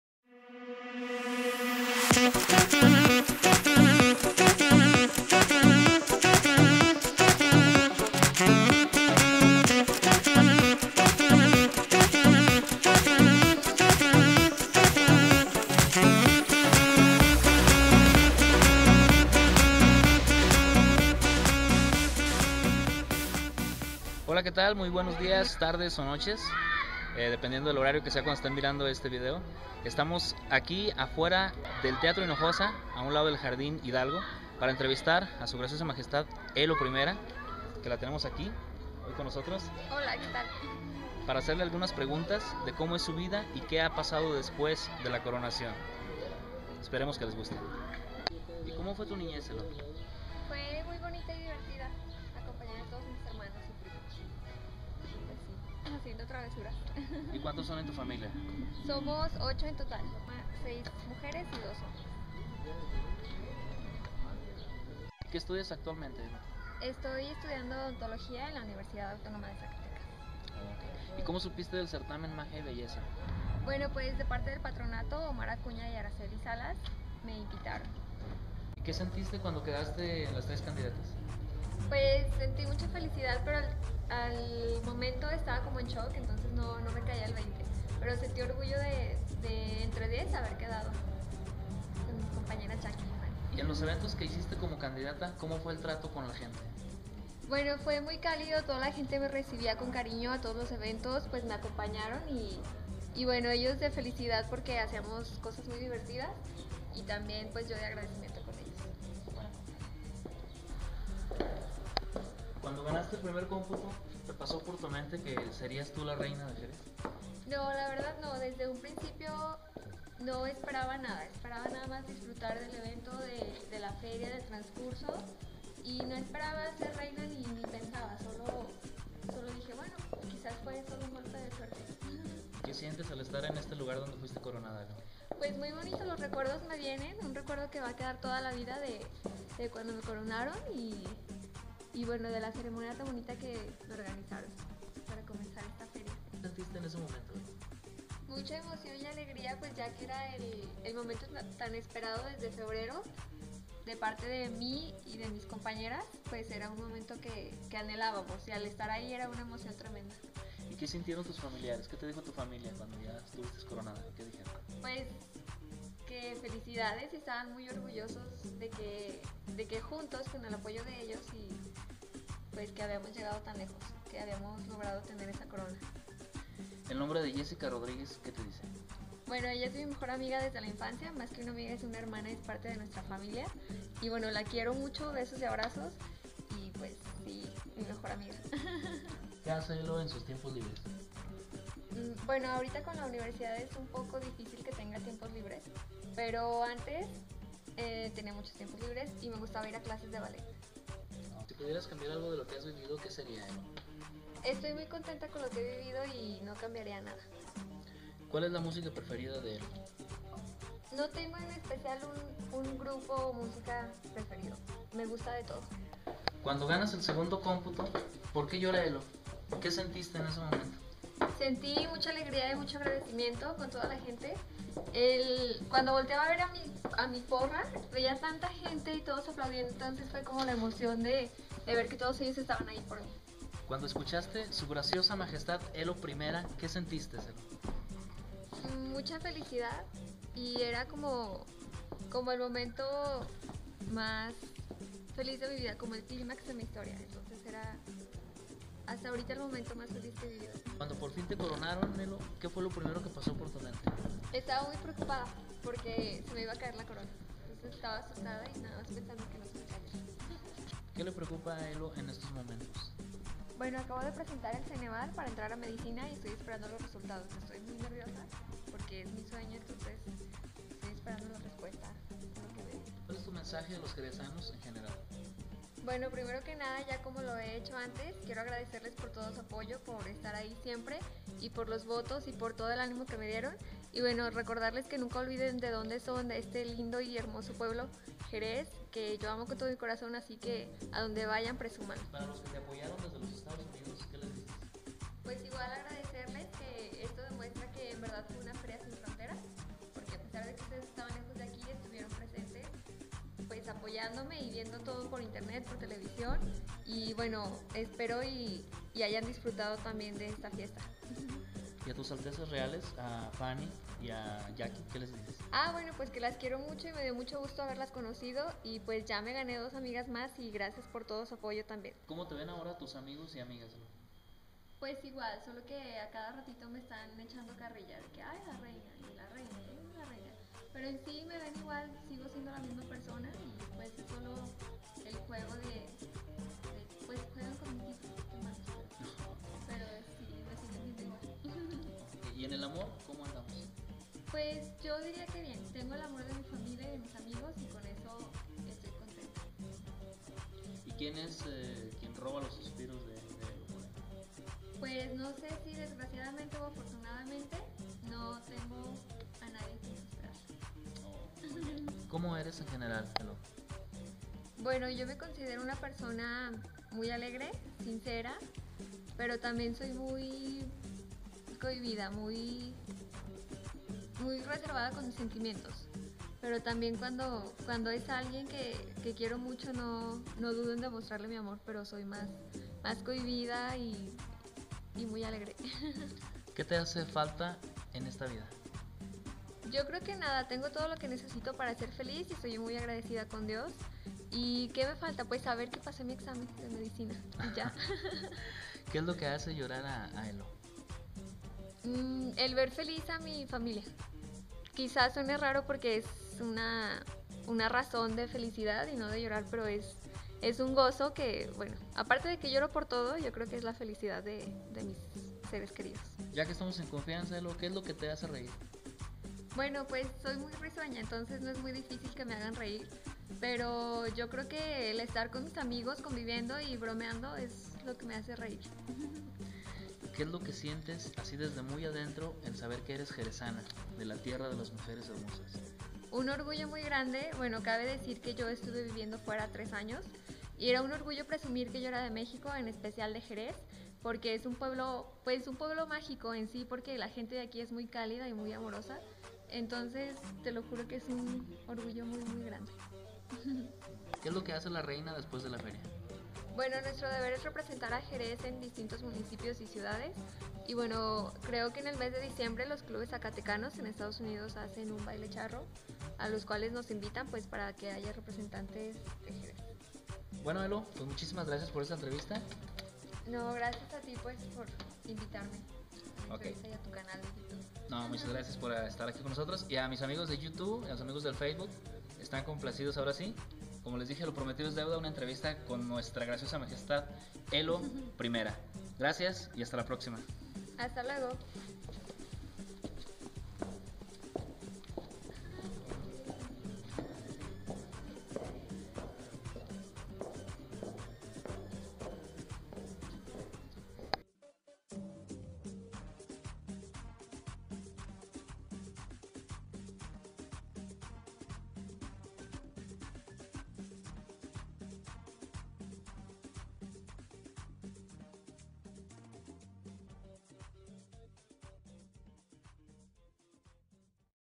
ENTREVISTA A UNA REINA ELO FLORES S.G.M. JEREZ ZACATECAS
AQUI UNA ENTREVISTA QUE LE HICE A LA REINA DE JEREZ HACE UNOS MESES  PARA QUE LA CONOZCAN  UN POCO MAS DE ESTA JEREZANA ESPERO Y LES GUSTE ATTE LUISMYMANGOREC Y GRACIAS POR MIRAR MI CANAL SALUDOS AMIGOS